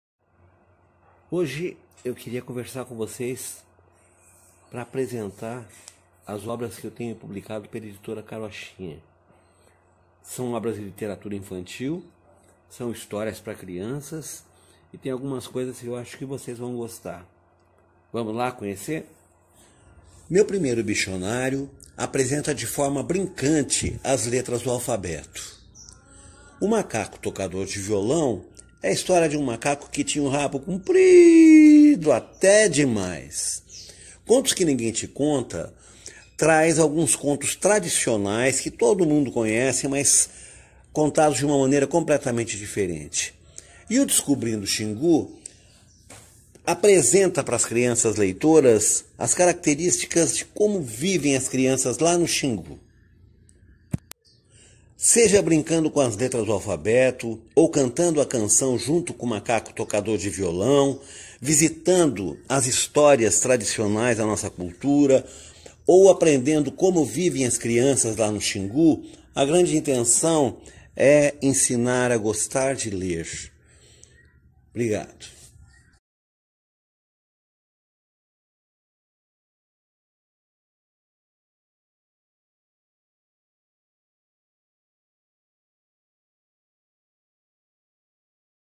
Publicações comentadas em 1 minuto
Vídeo apresentando capas e comentários sobre algumas obras publicadas pela Editora Carochinha com autoria do prof. Marco Hailer